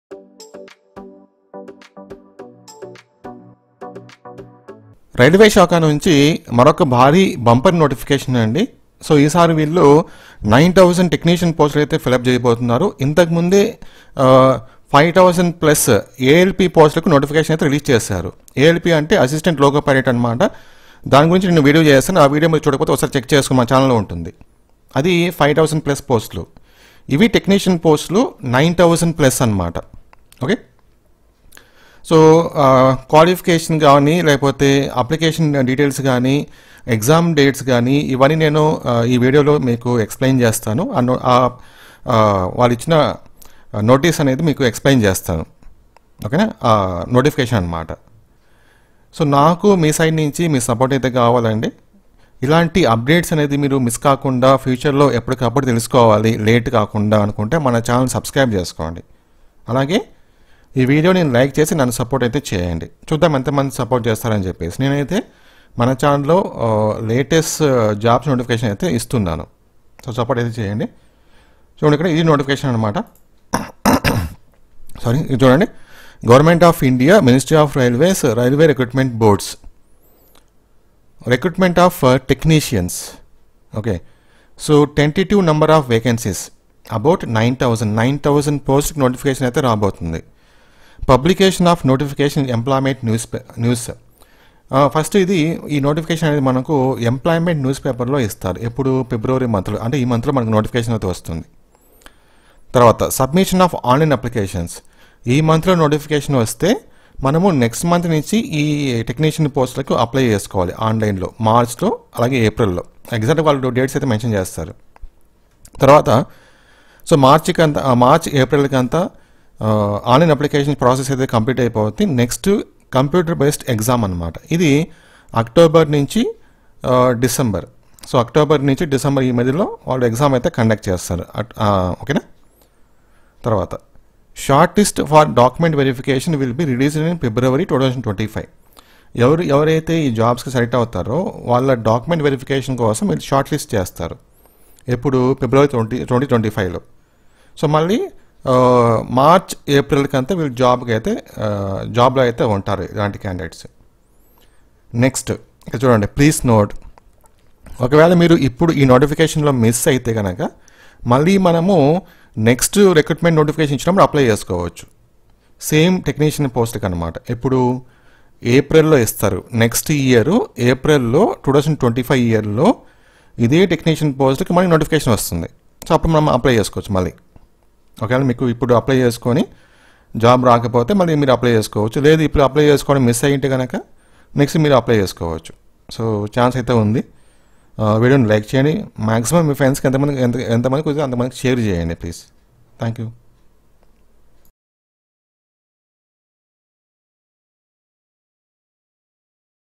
Railway New Vacancy Technicians Recruitment Notification 2024 Telugu | How To Apply Railway Jobs
Hi, welcome to the Teluguprapancham YouTube Channel.Railway New Vacancy Technicians Recruitment Notification 2024 Telugu | How To Apply Railway Jobs.